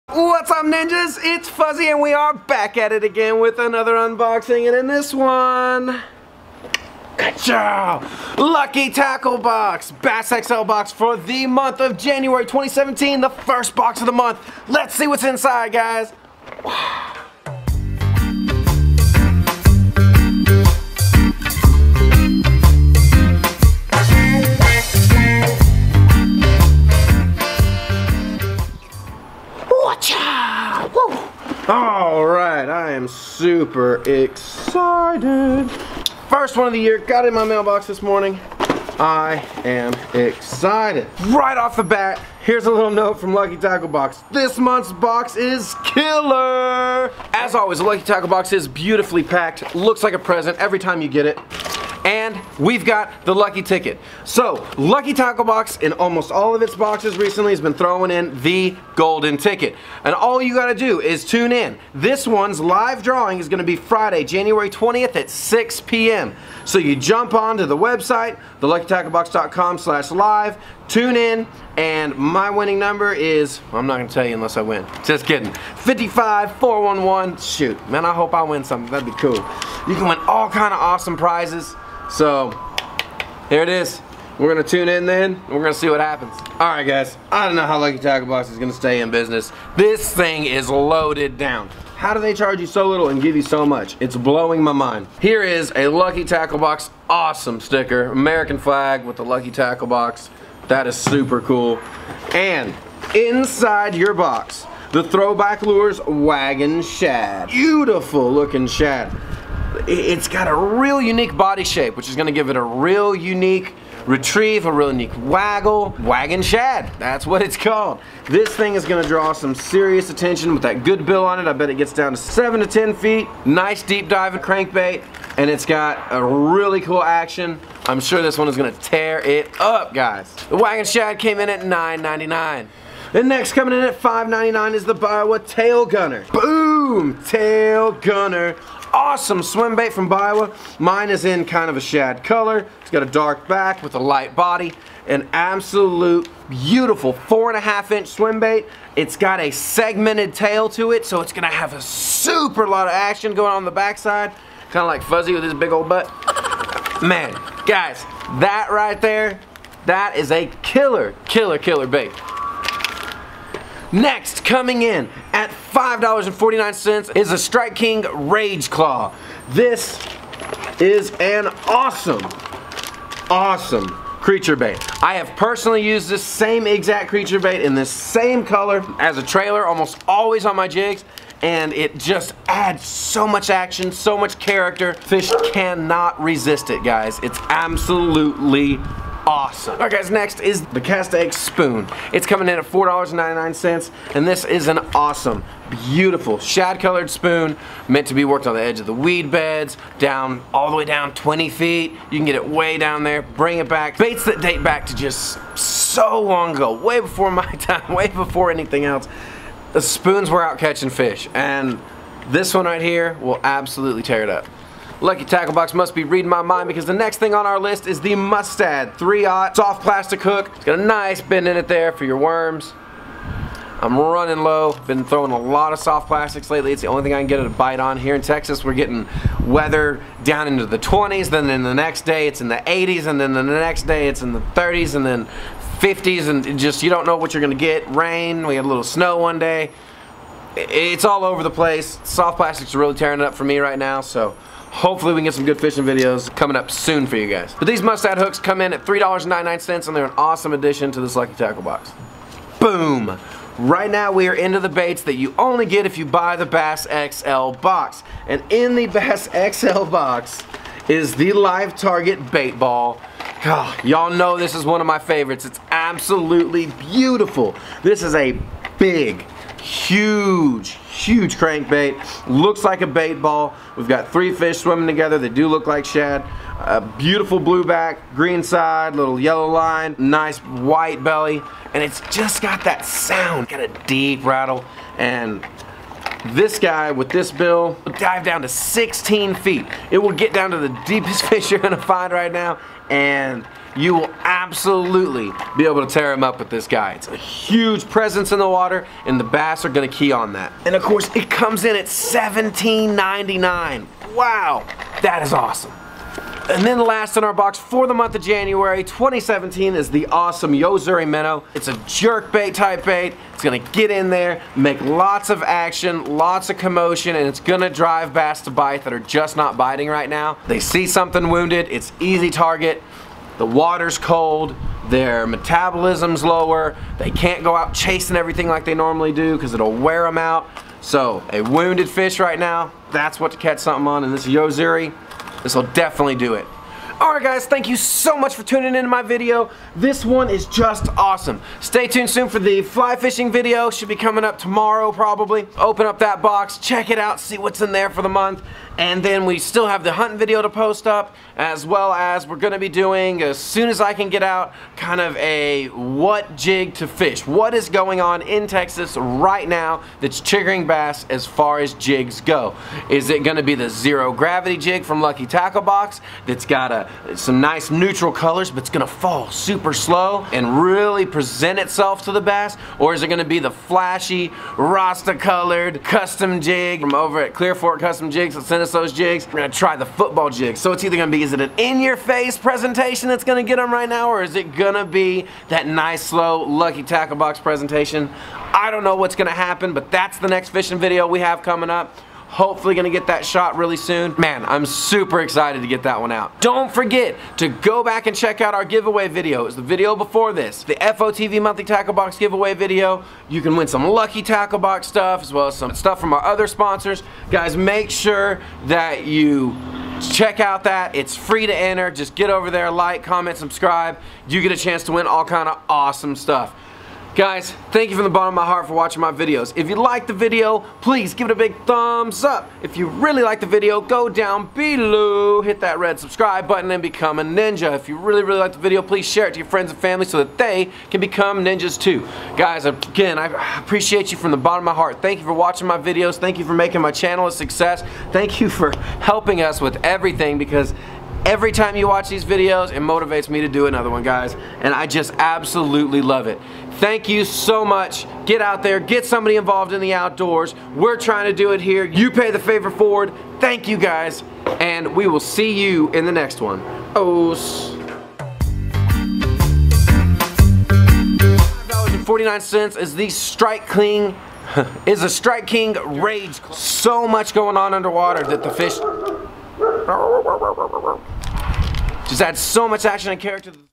Lucky Tackle Box Bass XL January 2017
Fuzzy unboxes the LTB Bass XL box for January 2017

Send your outdoor pics to us for a chance to be featured on the show and win an SA Fishing Face shield at